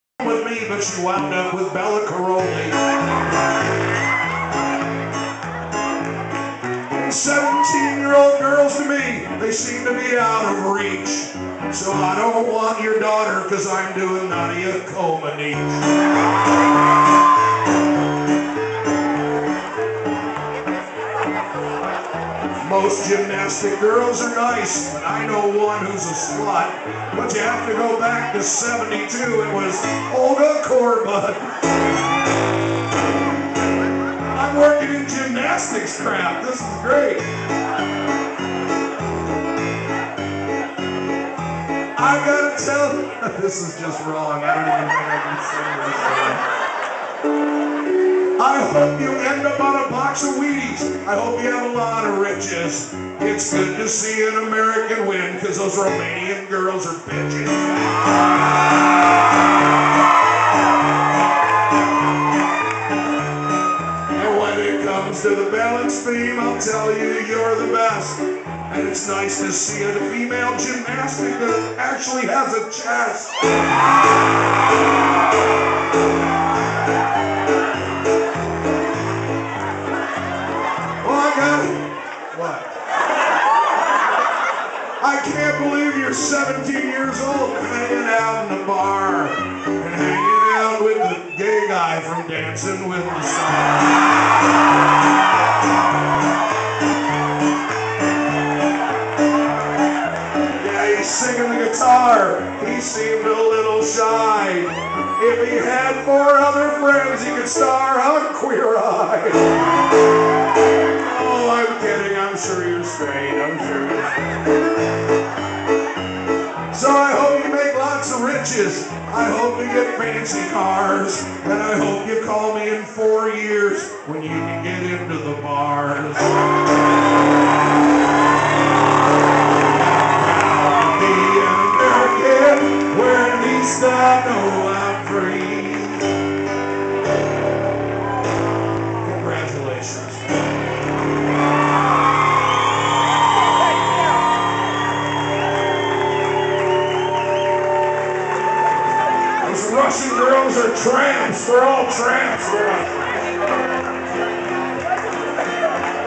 Shawn Johnson at Blue Moon
On April 17th, Shawn Johnson was at the Blue Moon Dueling Piano Bar in West Des Moines with her family and dance partner Mark Ballas from Dancing with the Stars, here is the video of her up on stage with the piano man singing a song to her. FUNNY!!!